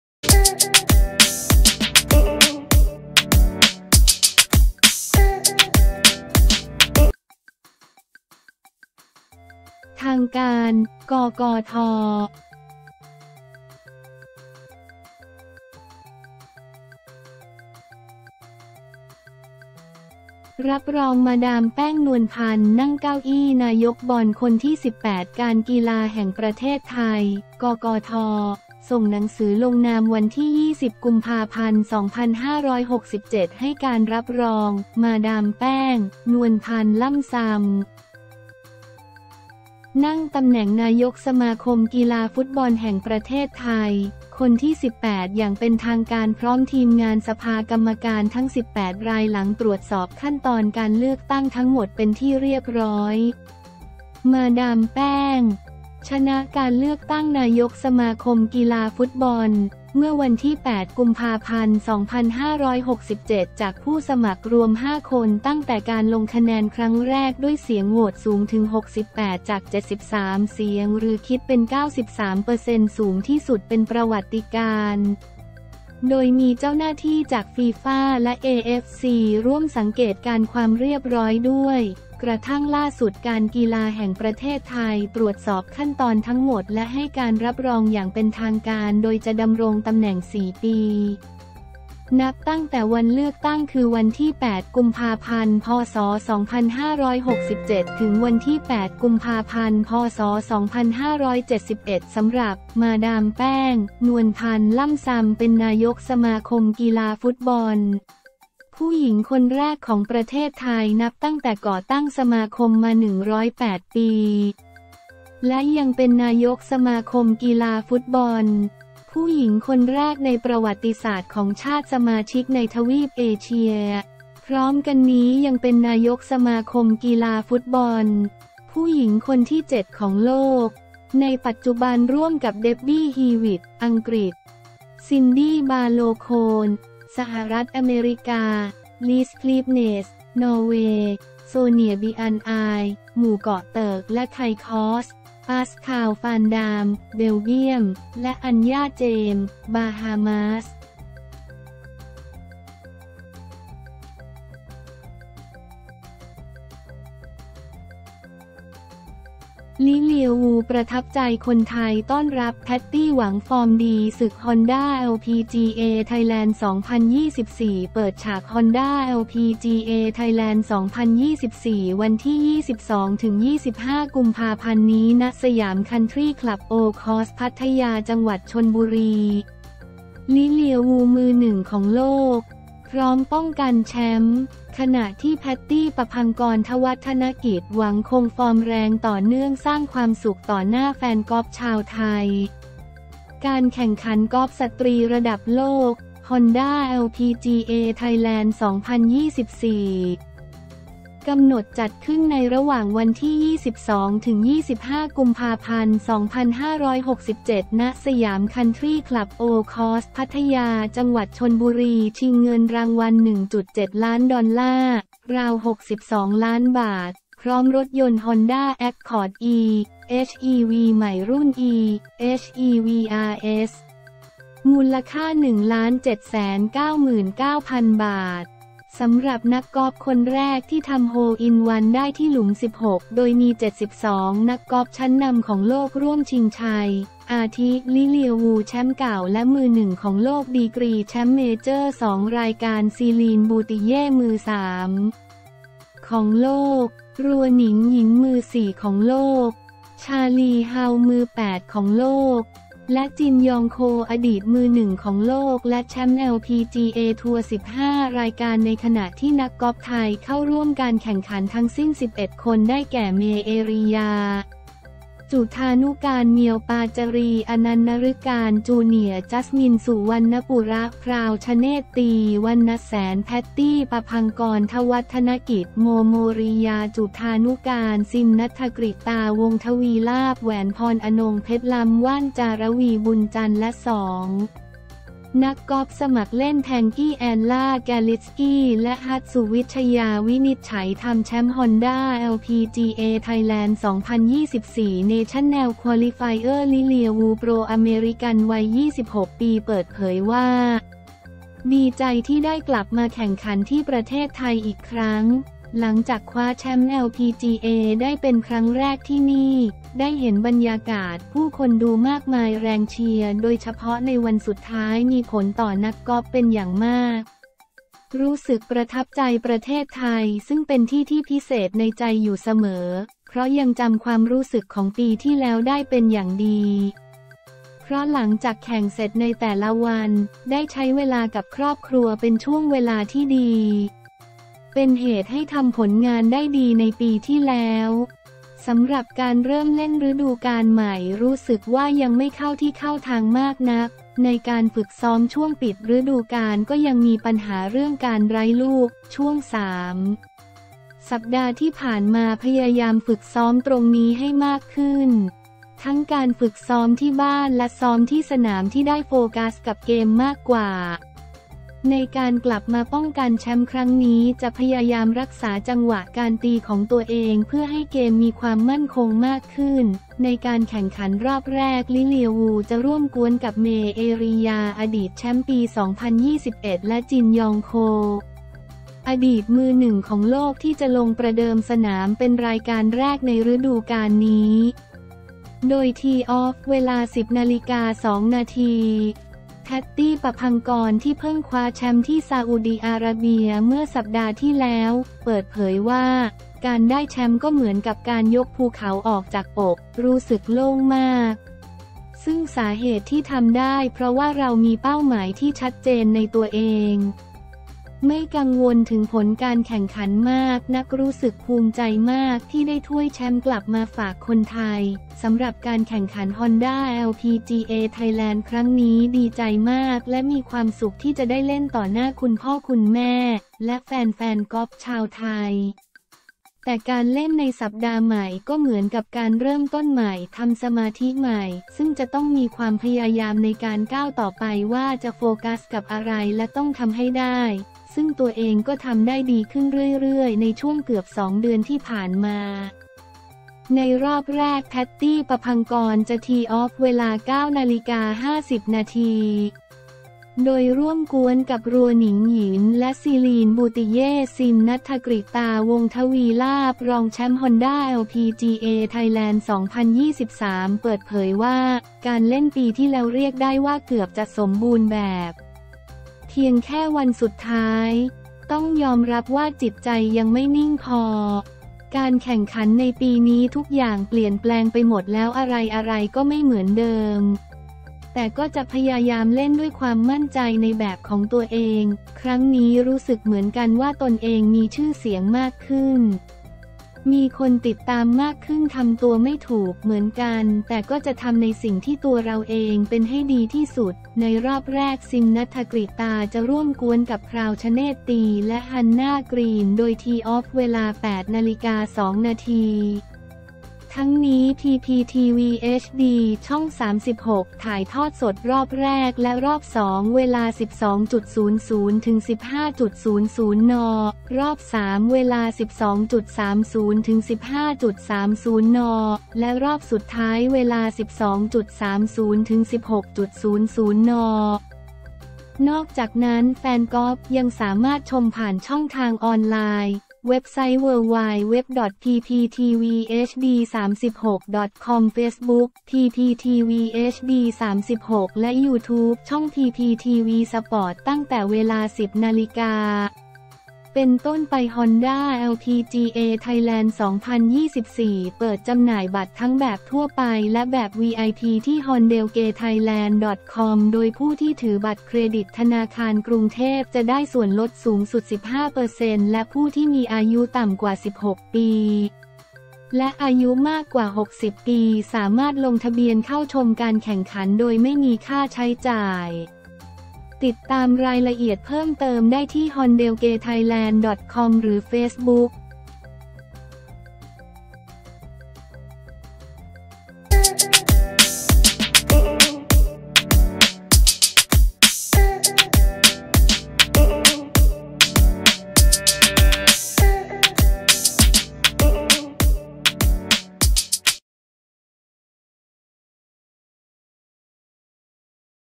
ทางการ! กกท รับรอง มาดามแป้ง นวลพรรณ นั่งเก้าอี้นายกบอล คนที่ 18
การกีฬาแห่งประเทศไทย (กกท.) ส่งหนังสือลงนาม วันที่ 20 กุมภาพันธ์ 2567 ให้การรับรอง “มาดามแป้ง” นวลพรรณ ล่ำซำ นั่งตำแหน่งนายกสมาคมกีฬาฟุตบอลแห่งประเทศไทยฯ คนที่ 18 อย่างเป็นทางการ พร้อมทีมงานสภากรรมการทั้ง 18 ราย หลังตรวจสอบขั้นตอนการเลือกตั้งทั้งหมดเป็นที่เรียบร้อย

“มาดามแป้ง” ชนะการเลือกตั้งนายกสมาคมกีฬาฟุตบอลฯ เมื่อวันที่ 8 กุมภาพันธ์ 2567 จากผู้สมัครรวม 5 คน ตั้งแต่การลงคะแนนครั้งแรก ด้วยเสียงโหวตสูงถึง 68 จาก 73 เสียง หรือ คิดเป็น 93% สูงที่สุดเป็นประวัติการณ์ โดยมีเจ้าหน้าที่จาก ฟีฟ่า และ เอเอฟซี ร่วมสังเกตการณ์ความเรียบร้อยด้วย กระทั่งล่าสุด การกีฬาแห่งประเทศไทยฯ ตรวจสอบขั้นตอนทั้งหมด และ ให้การรับรองอย่างเป็นทางการ โดยจะดำรงตำแหน่ง 4 ปี นับตั้งแต่วันเลือกตั้ง คือ วันที่ 8 กุมภาพันธ์ พ.ศ.2567 – 8 กุมภาพันธ์ พ.ศ.2571
สำหรับ “มาดามแป้ง” นวลพรรณ ล่ำซำ เป็นนายกสมาคมกีฬาฟุตบอลฯ ผู้หญิง คนแรก ของประเทศไทย นับตั้งแต่ก่อตั้งสมาคมฯ มา 108 ปี และ ยังเป็นนายกสมาคมกีฬาฟุตบอลฯ ผู้หญิง คนแรกในประวัติศาสตร์ ของชาติสมาชิกในทวีปเอเชีย พร้อมกันนี้ ยังเป็น นายกสมาคมกีฬาฟุตบอลฯ ผู้หญิง คนที่ 7 ของโลก ในปัจจุบัน ร่วมกับ เด็บบี้ ฮีวิตต์ (อังกฤษ) , ซินดี้ บาร์โลว์ โคน (สหรัฐฯ) , ลีซ คลีฟเนสส์ (นอร์เวย์) , โซเนีย บิอัน ไอม์ (หมู่เกาะเติก และ ไคคอส) , ปาสคาล ฟาน ดาม (เบลเบียม) และ อันย่า เจมส์ (บาฮามาส)